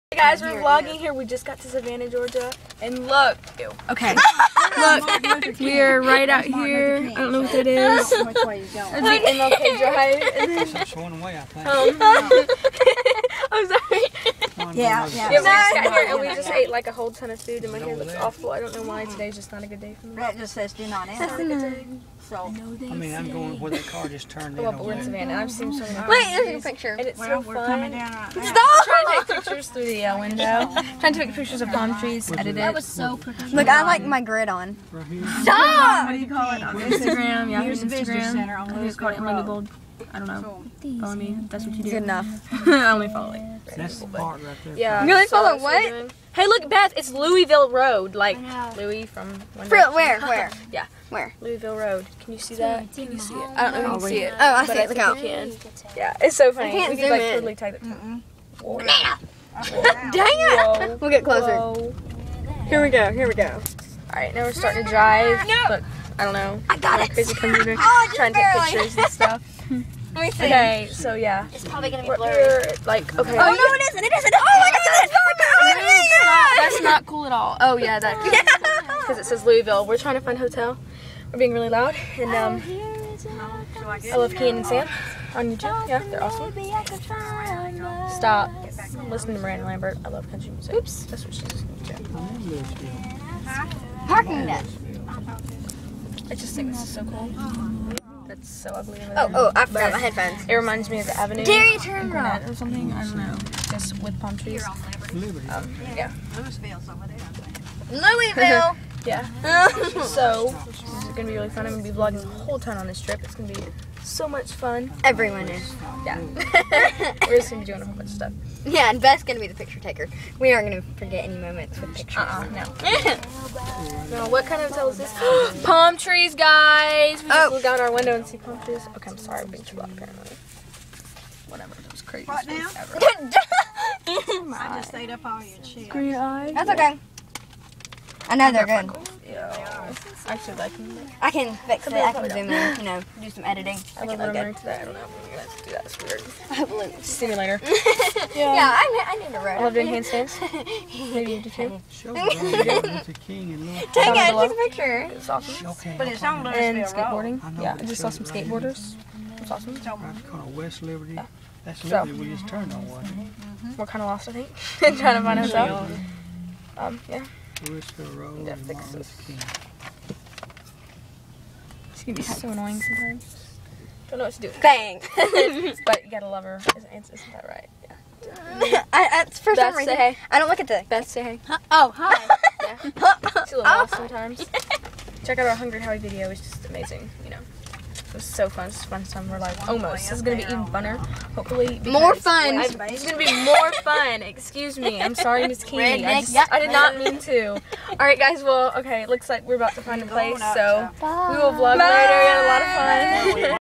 Car Vlog In Savannah, GA
We just arrived in Savannah, GA and felt like capturing the moment! I love Kian Lawley, Sam Potorff, and Marcus Butler!!!-3 just thought I'd let you know(;